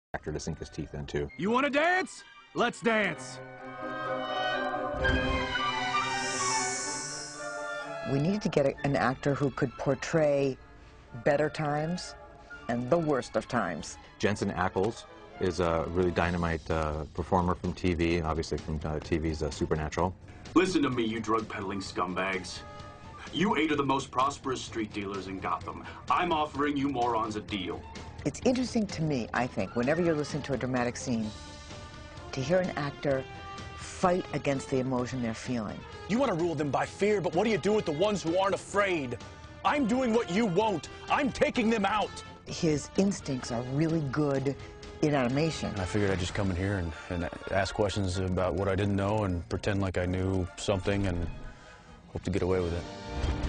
Jensen Ackles - Batman: Under the Red Hood
Jensen Ackles talks about his role as Red Hood in Batman: Under the Red Hood.